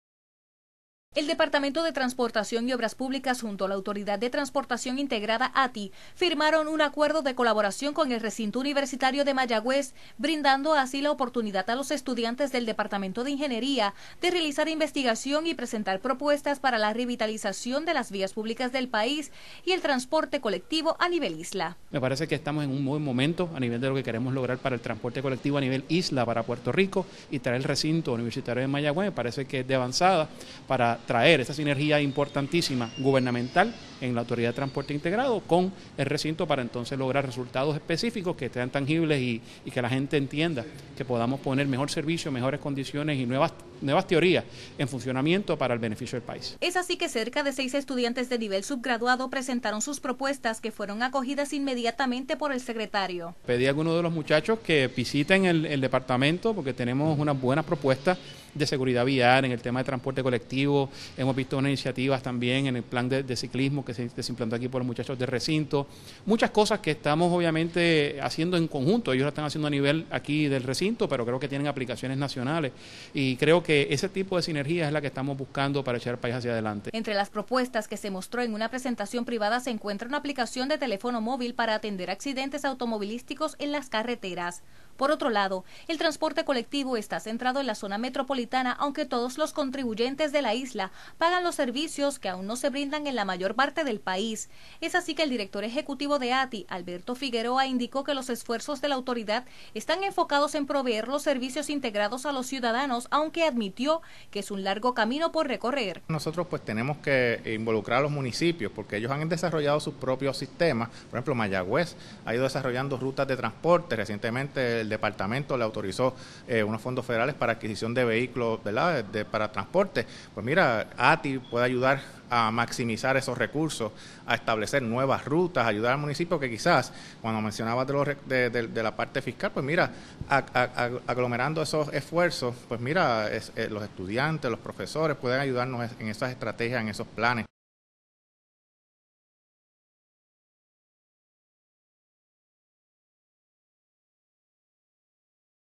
DTOP recibe propuestas de estudiantes del RUM
El Secretario del Departamento de Transportación y Obras Públicas escuchó varias propuestas de estudiantes del Departamento de Ingeniería del Recinto Universitario de Mayagüez para solucionar los problemas en las vías públicas del País. Se  firmó un  acuerdo entre la agencia y la universidad para que los estudiantes realicen estudios sobre el transporte colectivo dentro de la Autoridad de Transporte Integrado.